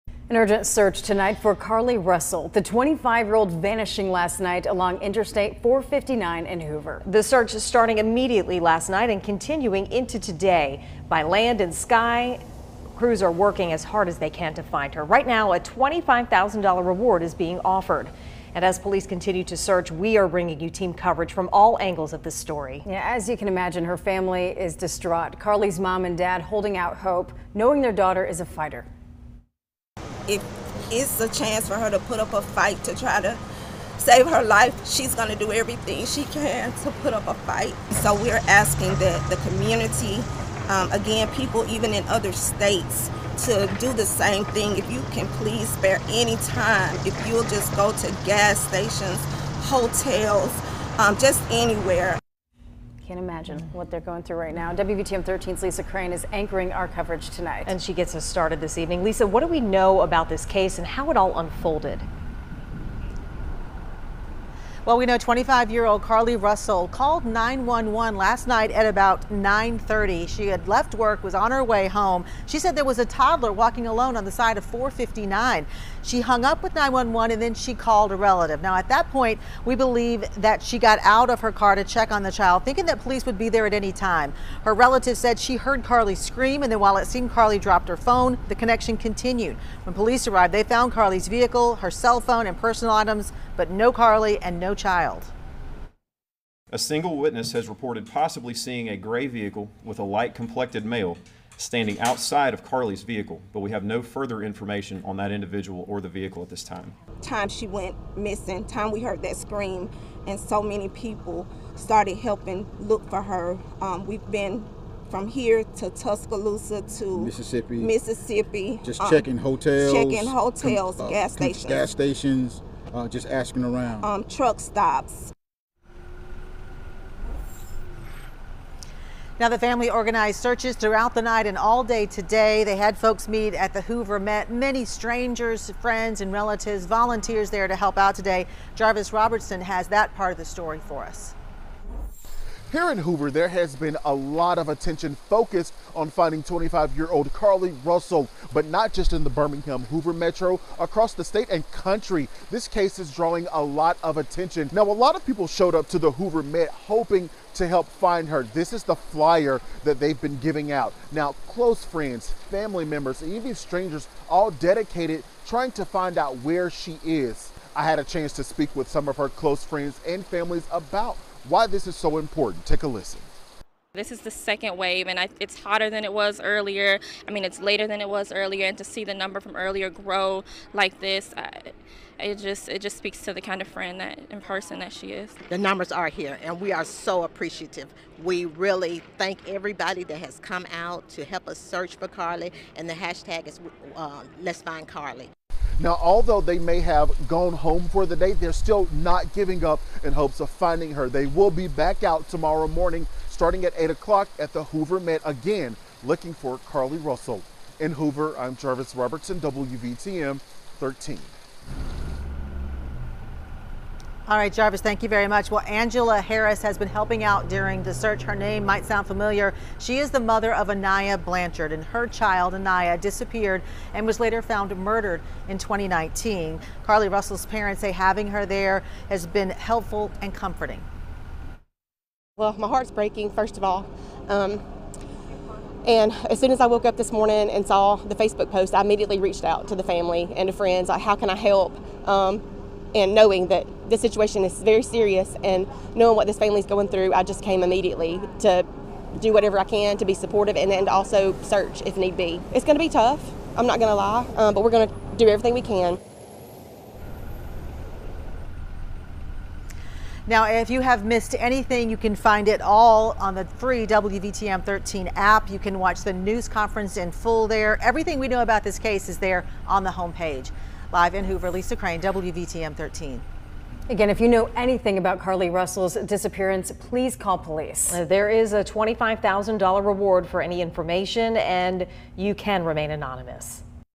'We're just gonna scour the earth': Search continues for missing woman who called 911 to report t...
'We're just gonna scour the earth': Search continues for missing woman who called 911 to report toddler along I-459